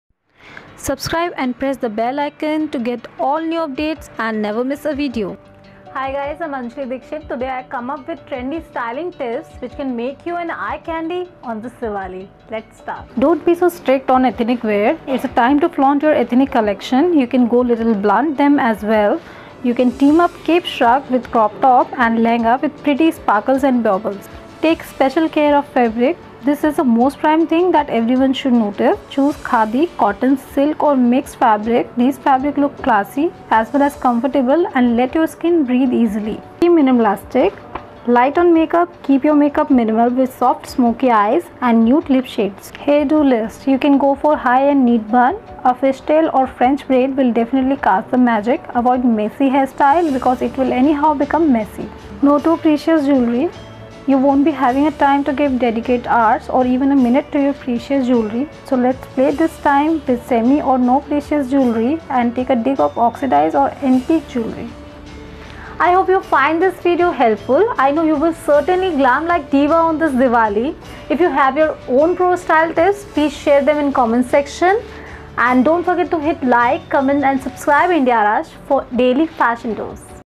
How to brighten your Diwali with best styling tips
When Diwali is on rounds, what strikes first in your head? About ladies, I am absolutely sure that it’s none other than shopping. Every Diwali, the fashionable designer dress is what they need most.